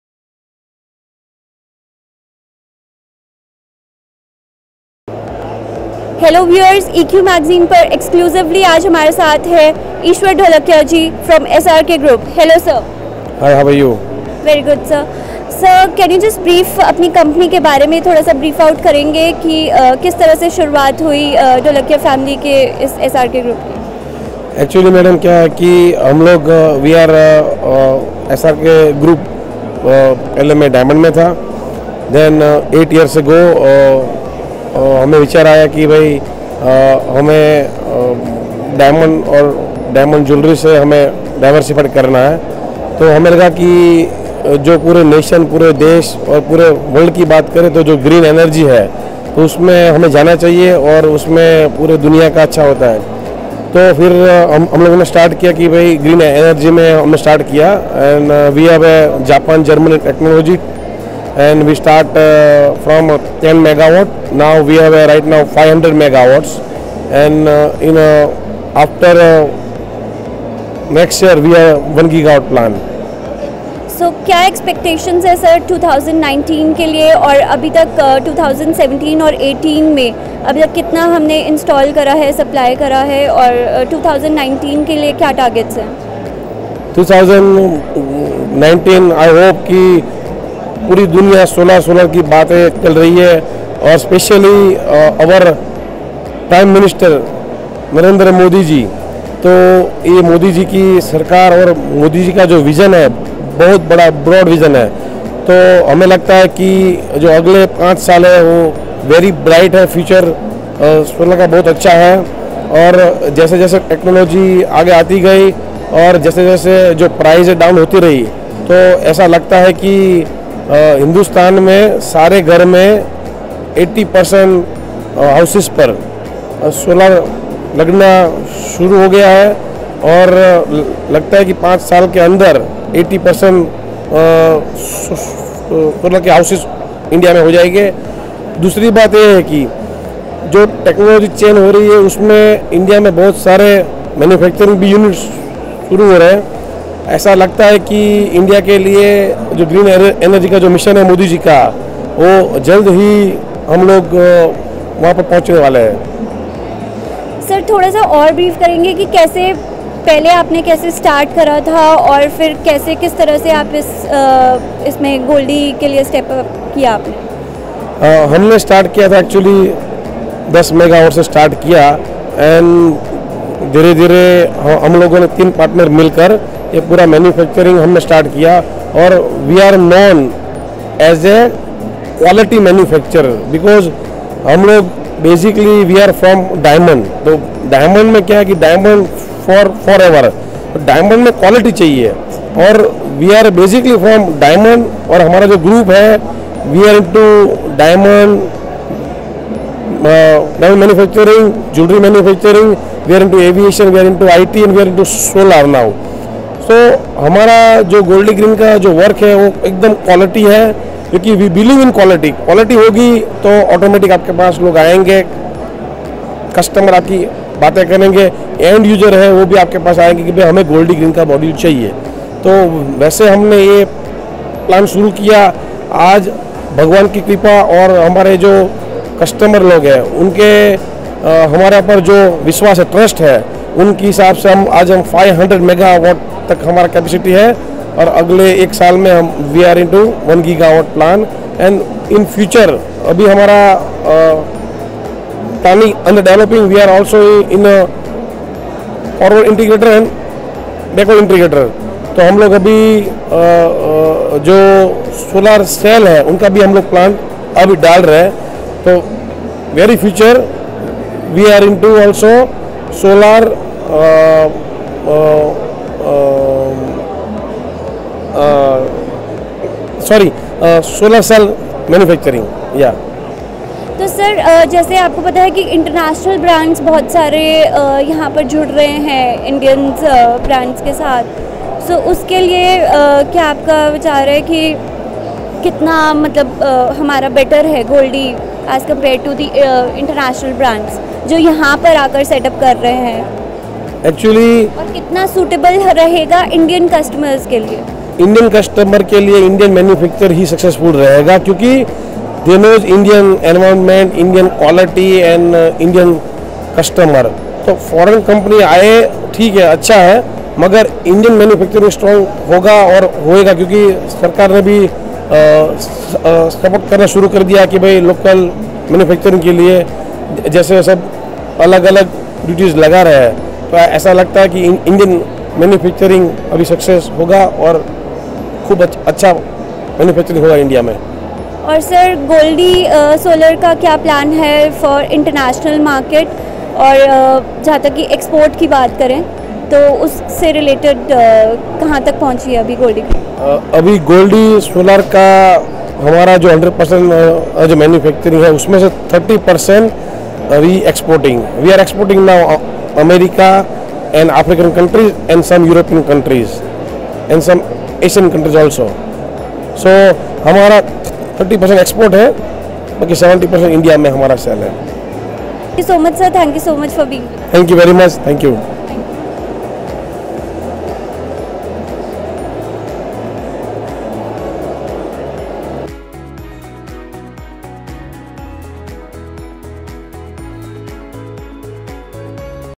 EQ in conversation with Mr. Ishver Dholakiya Capt. Managing Director at GOLDI
GOLDI Officials in conversation with EQ Magazine at REI- 2018,
At India Exposition Mart Limited, Greater Noida, Uttar Pradesh